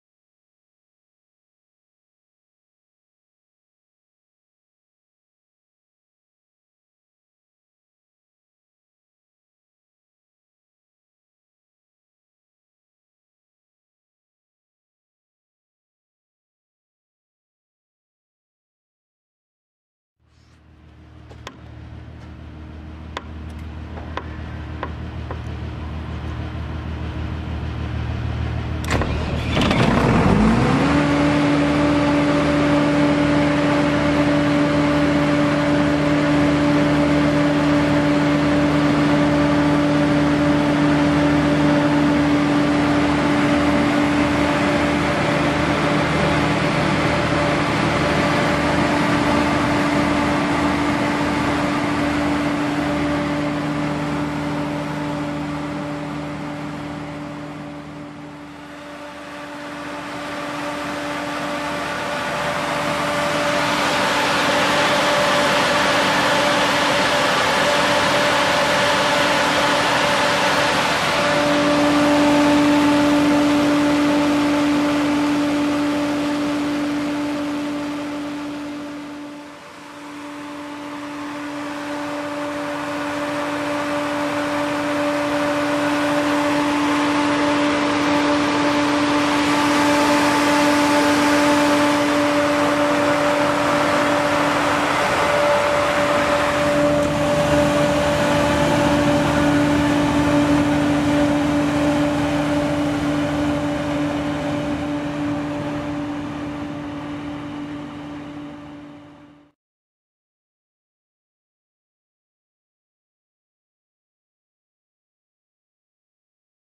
Pramac GSW170 2007
This second handed PRAMAC - GSW170 is in a good working condition and could be used directly. Pfeifer Heavy Machinery cares about organizing transportation, all necessary documents and everything related to the process of buying a used machine.

Manufacturer: Pramac
Typ: GSW170
Building year: 2007
Fuel: Diesel
Serial number: PEE2408730
Working hours: 12.460 h
Engine manufacture / typ: Deutz / BF 6M 1013 EC
Engine number: 30S10474424
Altinator: Stamford UCI 274 F
Capacity in KW: 153
Capacity in HP: 205
Capacity in kVA: 160
Rated voltage: 400
Frequency: 50 Hz
COSPHi: 0.8